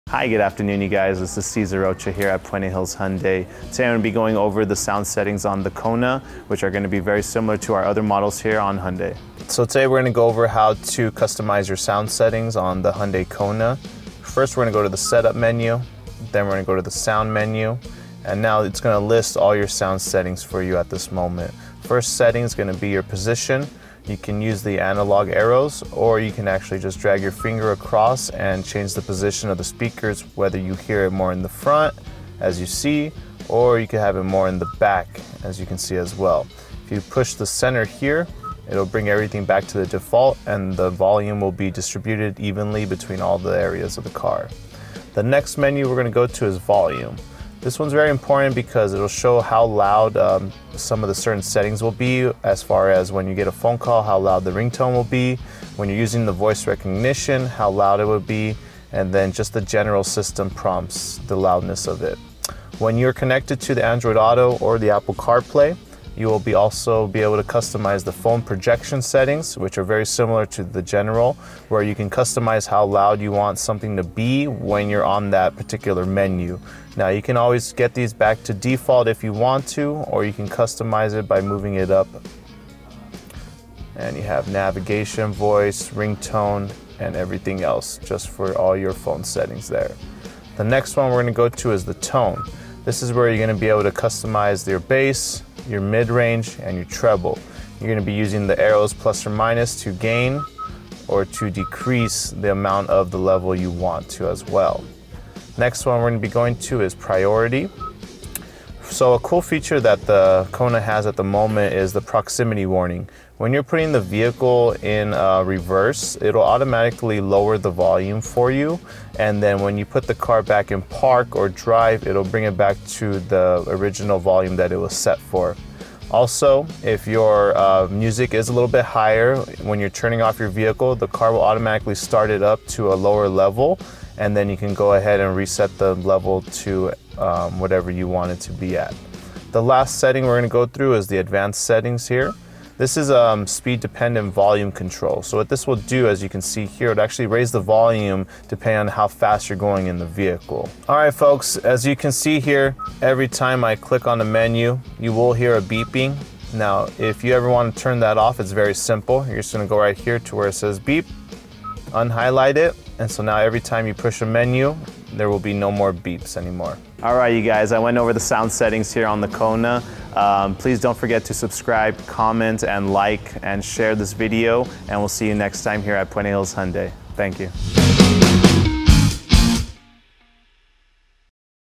2019 Hyundai Kona | Sound System Explained
So you're interested in the Kona & wondering how your music would sound like, or how to even use the full capabilities of the Infinity sound system? Cesar will show you exactly just how 😎